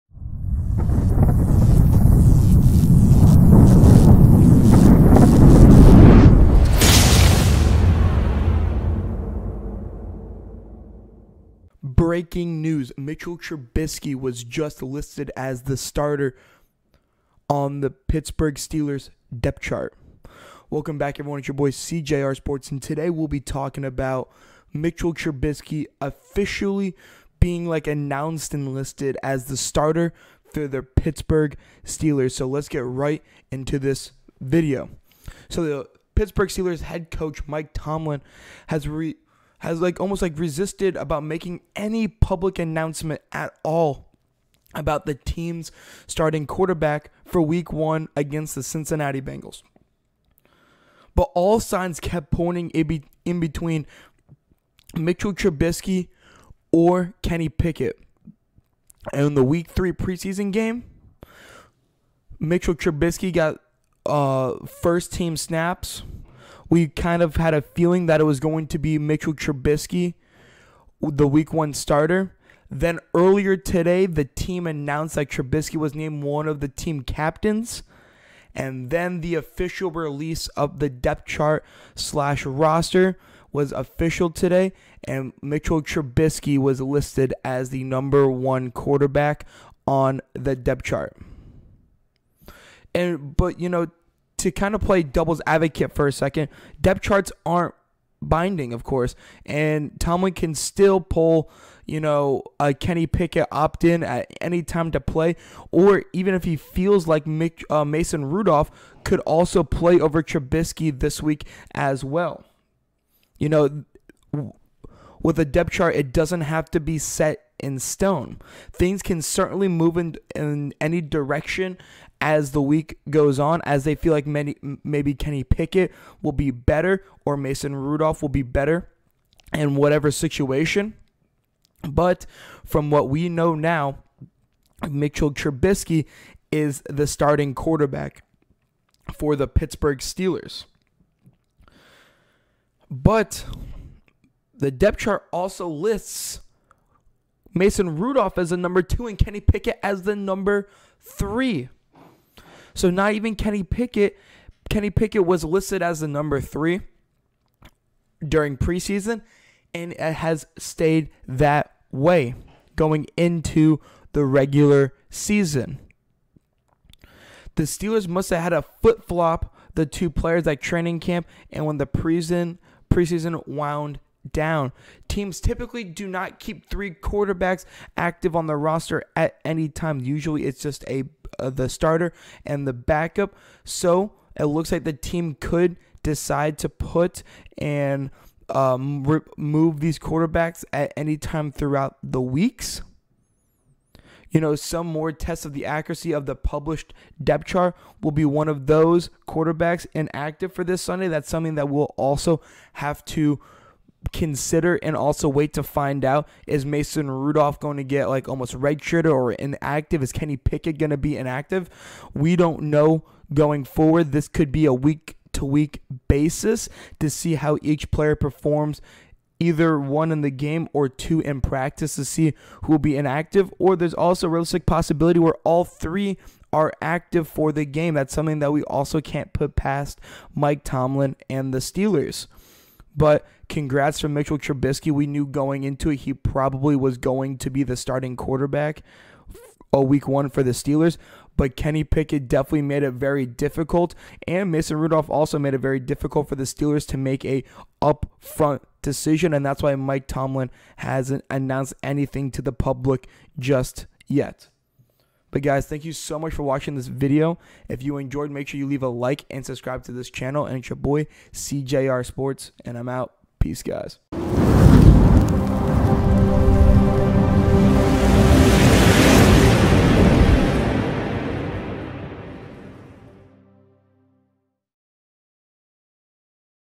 Breaking News: Mitch Trubisky is the starting quarterback for the Pittsburgh Steelers!
The PittsburghSteelers head coach Mike Tomlin has resisted making any public pronouncements about the team’s starting quarterback for their Week 1 game against the Bengals, but the signs keep pointing to MitchTrubisky getting the first shot under center. The team announced that Trubisky has been named one of their captains on Monday and they also released a depth chart for the first week of the season a bit later in the day. It lists Trubisky as the No. 1 quarterback. Depth charts aren’t binding, of course, and Tomlin could still opt to play Kenny Pickett or Mason Rudolph over Trubisky this weekend, but it certainly seems like thinks are moving in Trubisky’s direction with the start of the regular season days away. The depth chart lists Rudolph as the No. 2 with Pickett at No. 3 after it looked like the Steelers had flip-flopped the two players as training camp and the preseason wound down. Teams typically don’t keep three quarterbacks active, so one more test of the accuracy of the published depth chart will be if one of those quarterbacks is inactive this Sunday for the Steelers.